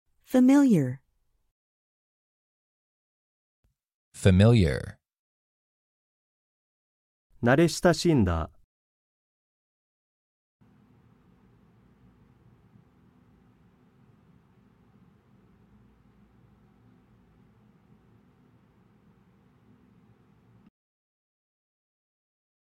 英語で"慣れ親しんだ"の発音の仕方
男性コーチ
Familiar

女性コーチ
Familiar

慣れ親しんだ

慣れ親しんだ